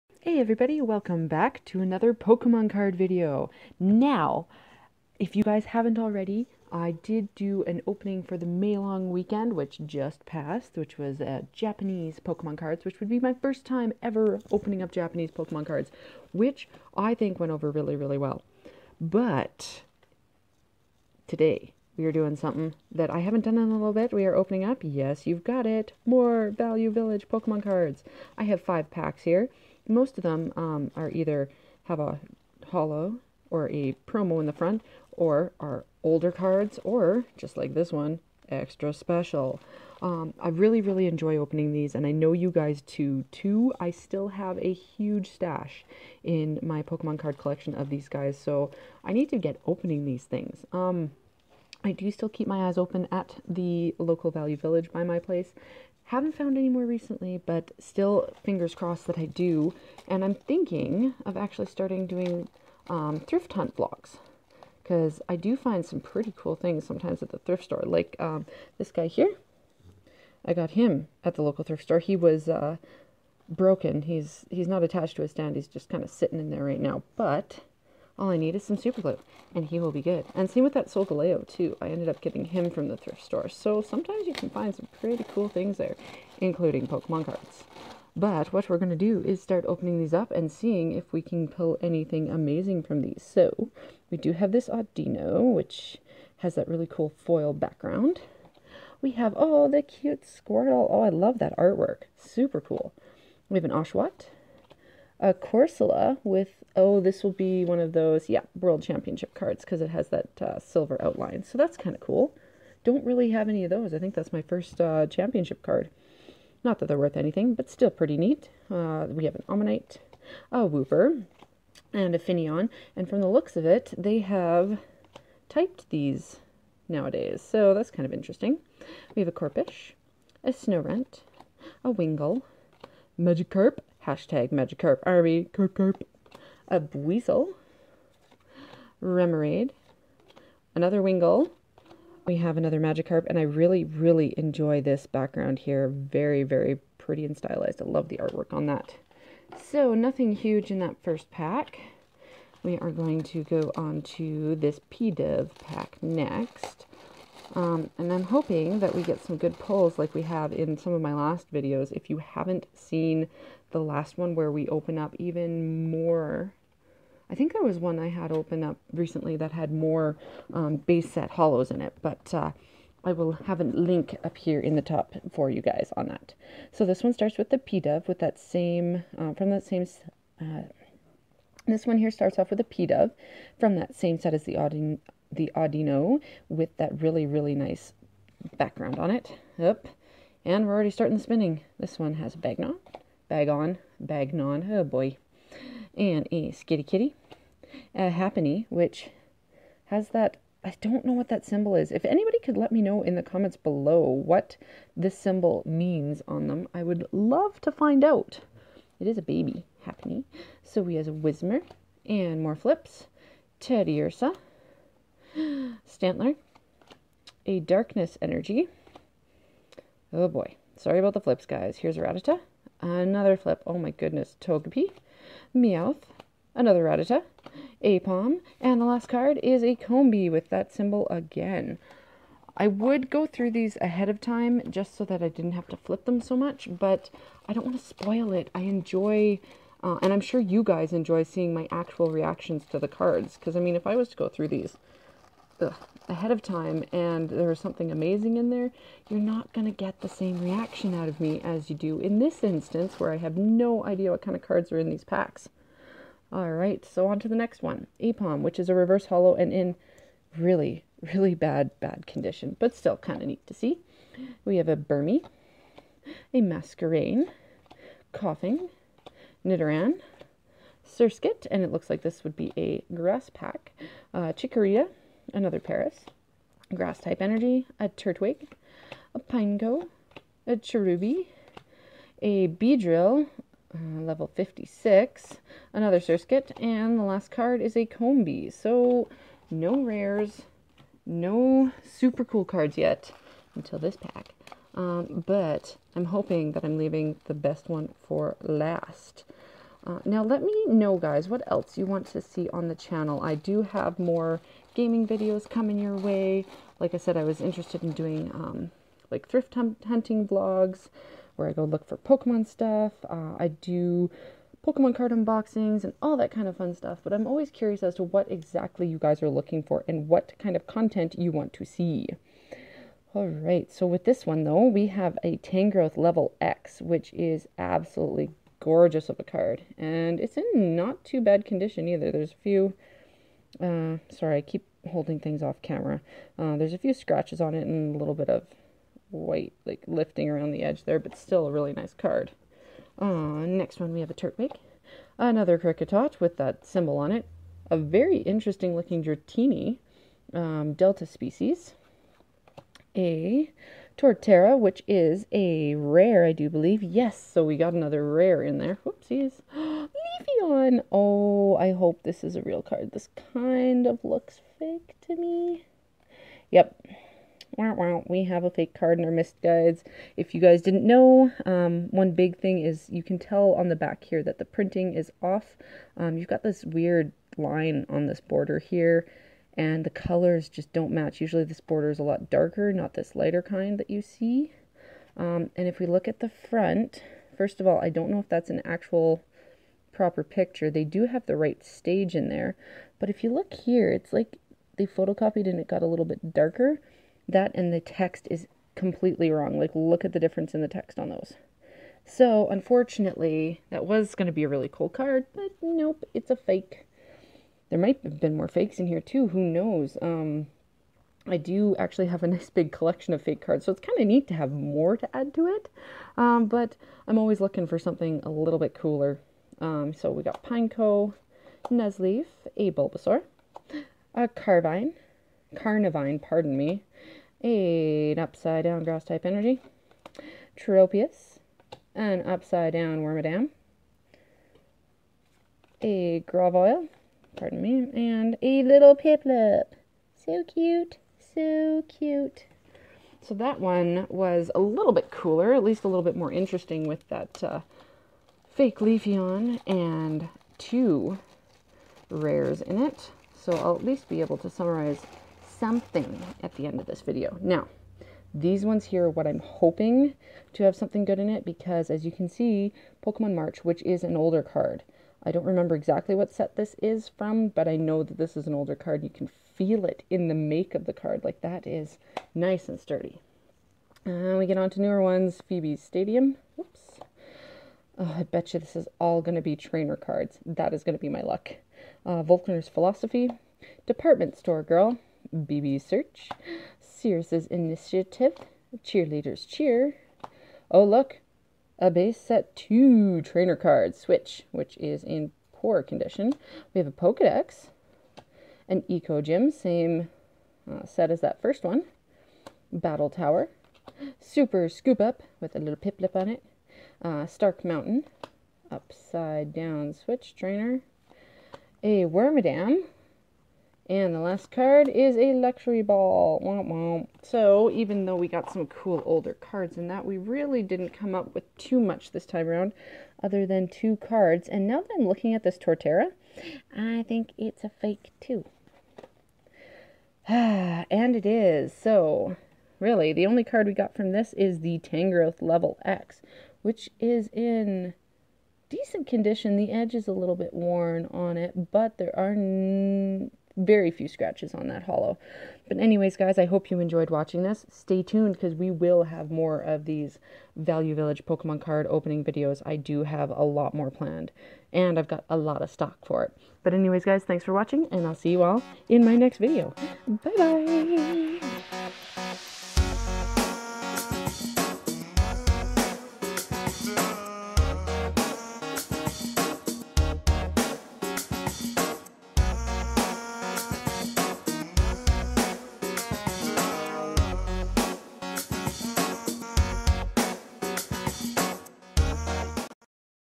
Thrift Store Pokemon Cards - Another Mixed Bag! Part 5
Today we dig into another bunch of thrift store pokemon cards. The front of these look promising with some holo's, promo's and older cards; but sometimes looks can be deceiving! We have five more packs that came from my local Value Village at $2.99 CAD each. I really enjoy opening these up as you never know what you're going to get. You can (and I have) get some amazing cards when people donate their cards. Half the fun is the chance of getting nothing or something awesome!